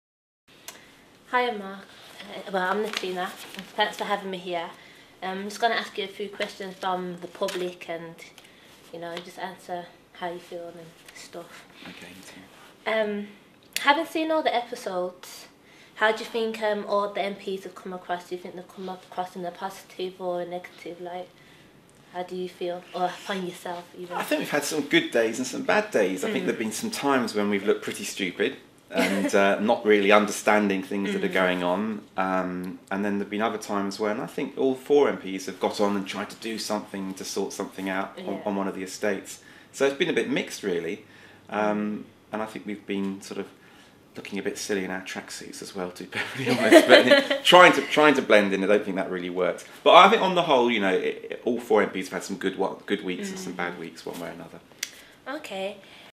Tower Block of Commons MP Mark Oaten interviewed by Natina James.
Tower Block of Commons MP Mark Oaten is interviewed by Natina James on behalf of Yoosk.com. Here he is asked a question about how he felt the MPs came across in the show.